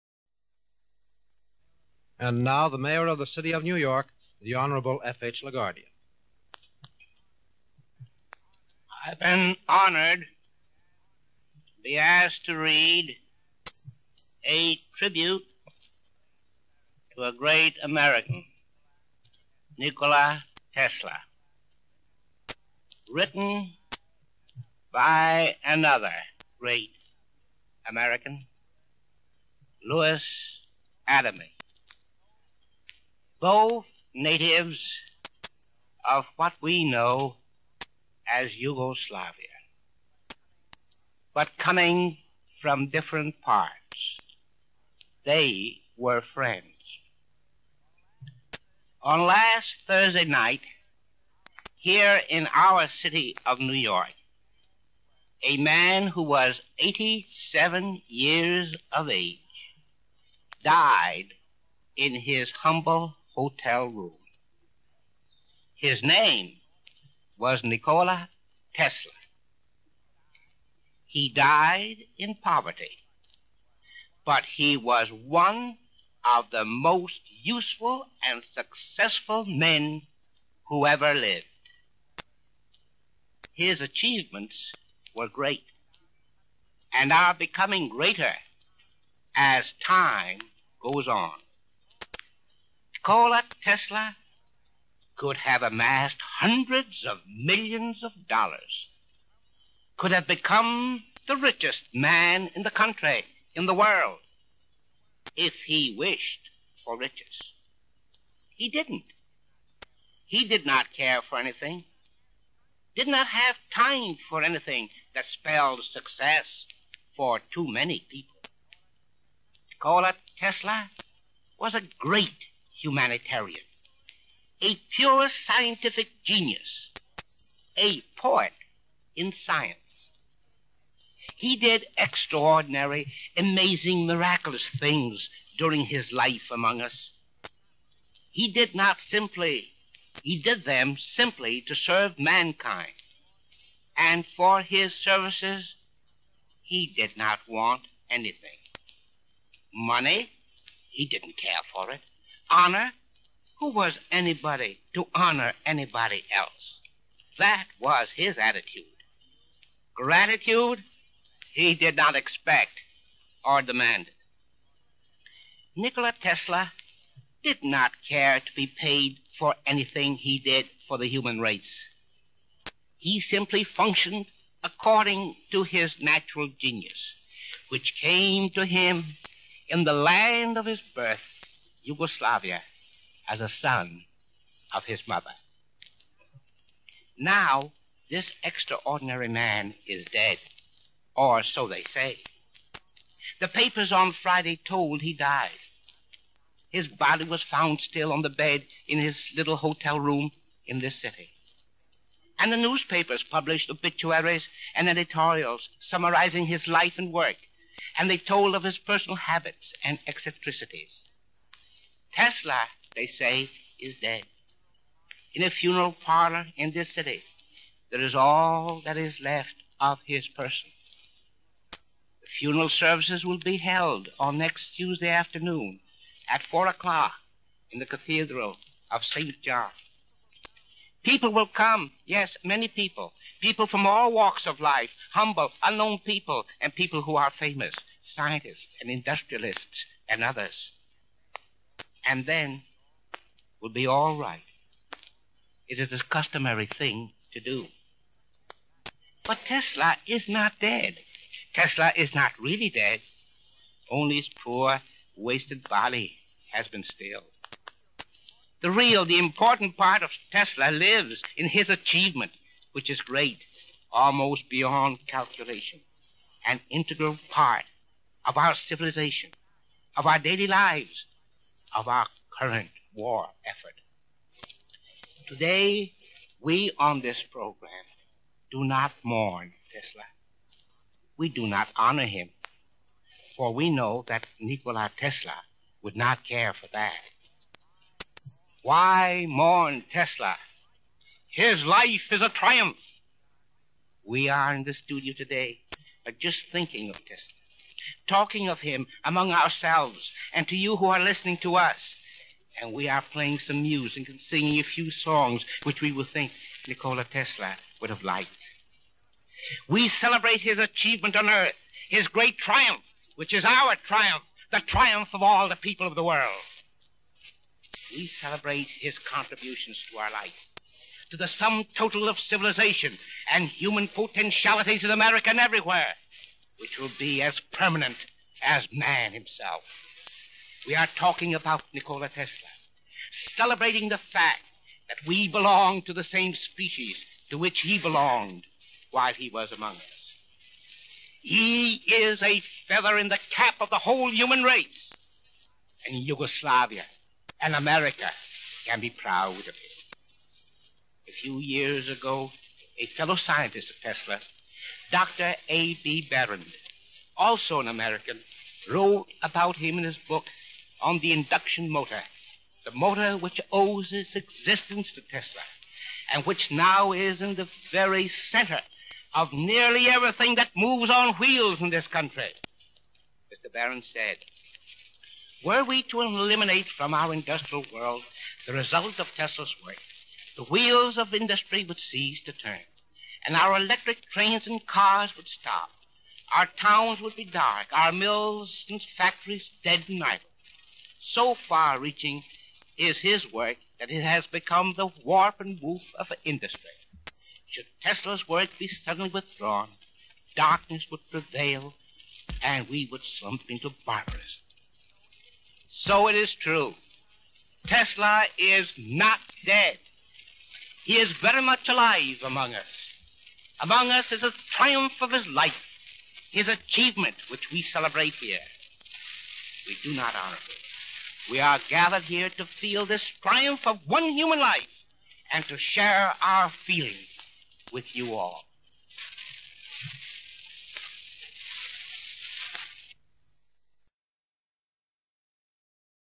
Nikola Tesla Eulogized
Mayor of the City of New York, Fiorello Laguardia, delivered a eulogy to Tesla broadcast on New York Radio, January 10, 1943.

And now the Mayor of the city of New York, the Honorable F.H. LaGuardia:

I have been honored and been asked to read a tribute to a great American, Nikola Tesla, written by another great American, Lewis Adaleigh, both natives of what we know as Yugoslavia but coming from different parts. They were friends.

On last Thursday night here in our city of New York, a man who was 87 years of age died in his humble hotel room. His name was Nikola Tesla. He died in poverty, but he was one of the most useful and successful men who ever lived. His achievements were great and are becoming greater as time goes on. Nikola Tesla could have amassed hundreds of millions of dollars, could have become the richest man in the country, in the world, if he wished for riches. He didn't. He did not care for anything, did not have time for anything that spelled success for too many people.

Nikola Tesla was a great humanitarian, a pure scientific genius, a poet in science. He did extraordinary amazing miraculous things during his life among us. He did them simply to serve mankind and for his services, he did not want anything. Money, he didn't care for it. Honor, who was anybody to honor anybody else. That was his attitude. Gratitude, he did not expect or demand. Nikola Tesla did not care to be paid for anything he did for the human race. He simply functioned according to his natural genius which came to him in the land of his birth, Yugoslavia, as a son of his mother.

Now this extraordinary man is dead or so they say. The papers on Friday told he died, his body was found still on the bed in his little hotel room in this city and the newspapers publish obituaries and editorials summarizing his life and work and told of his personal habits and eccentricities. Tesla they say is dead. In a funeral parlor in this city there is all that is left of his person. The funeral services will be held on next Tuesday afternoon at 4:00 in the Cathedral of Saint John. People will come. Yes, many people, people from all walks of life, humble unknown people and people who are famous scientists and industrialists and others and then we'll be all right. It is a customary thing to do, but Tesla is not dead.

Tesla is not really dead. Only his poor wasted body has been stilled. The real, the important part of Tesla lives in his achievement which is great, almost beyond calculation, an integral part of our civilization, of our daily lives, of our current war effort. Today, we, on this program, do not mourn Tesla. We do not honor him for we know that Nikola Tesla would not care for that. Why mourn Tesla? His life is a triumph. We are in the studio today just thinking of Tesla, talking of him among ourselves and to you who are listening to us, and we are playing some music and can sing a few songs which will think Nikola Tesla would have liked.

We celebrate his achievement on earth, his great triumph which is our triumph, the triumph of all the people of the world. We celebrate his contributions to our life, to the sum total of civilization and human potentialities to Americans everywhere which will be as permanent as man himself. We are talking about Nikola Tesla, celebrating the fact that we belong to the same species to which he belonged while he was among us. He is a feather in the cap of the whole human race and Yugoslavia and America can be proud of him.

A few years ago, a fellow scientist of Tesla, Dr. A.B. Baron, also an American, wrote about him in his book on the induction motor, the motor which owes its existence to Tesla and which, now, is in the very center of nearly everything that moves on wheels in this country. Mr. Baron said were we to eliminate from our industrial world the result of Tela's work, the wheels of industry would cease to turn and our electric trains and cars would stop, our towns would be dark, our mills and factories dead. So far reaching is his work that it has become the warp and whoop of industry. Should Tela's work be suddenly withdrawn, darkness would prevail and we would slump into darkness.

So it is true, Tesla is not dead. He is very much alive among us. Among us is a triumph of his life, his achievement which we celebrate here. We do not honor him. We are gathered here to feel this triumph of one human life and to share our feelings with you all.

Nikola Tesla was a man of the future, always thinking ahead of his time. He predicted interplanetary communication and death rays that would make America impregnable from land, air and sea and war impossible.